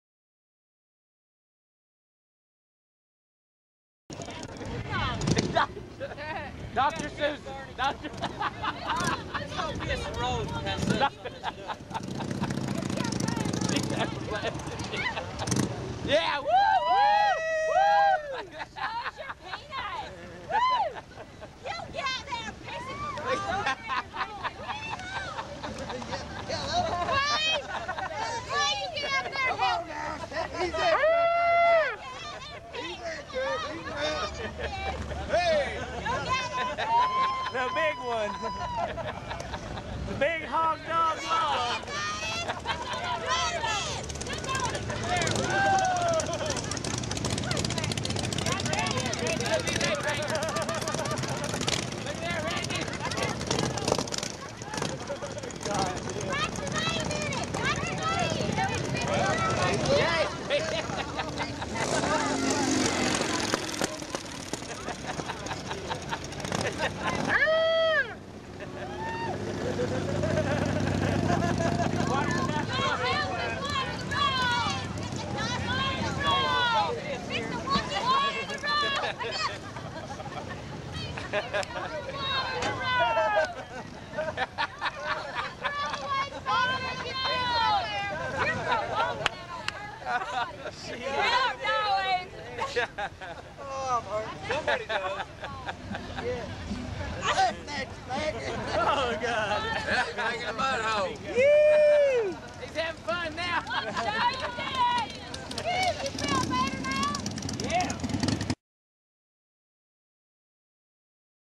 Mack Pee's Forever
Semo Biker Rally 93.Mack's feel'in no pain. It's Dusty,He's got to pee. What would you do?